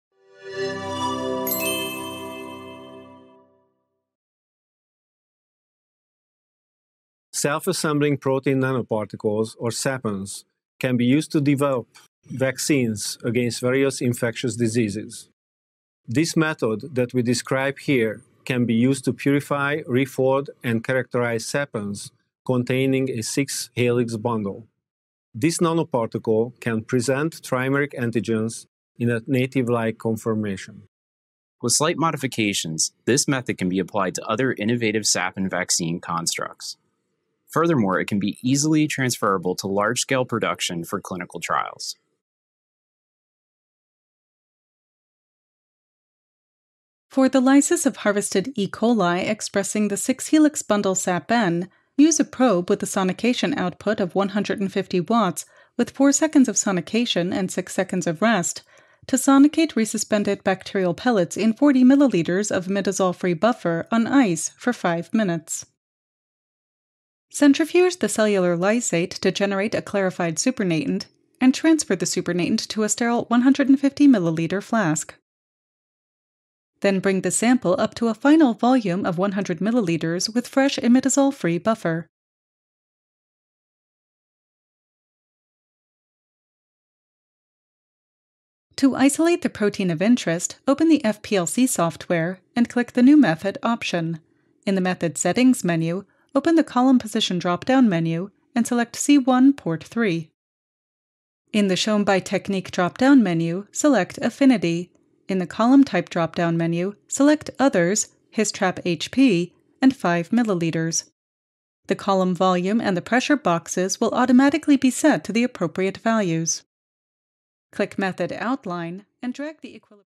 Production-E. coli-Expressed Self-Assembling Protein Nanoparticles l Protocol Preview
Production of E. coli-expressed Self-Assembling Protein Nanoparticles for Vaccines Requiring Trimeric Epitope Presentation -  a 2 minute Preview of the Experimental Protocol

Christopher  P. Karch, Peter Burkhard, Gary  R. Matyas, Zoltan Beck
Walter Reed Army Institute of Research, U.S. Military HIV Research Program; Henry M. Jackson Foundation for the Advancement of Military Medicine,; Alpha-O Peptides AG,;

A detailed method is provided here describing the purification, refolding, and characterization of self-assembling protein nanoparticles (SAPNs) for use in vaccine development.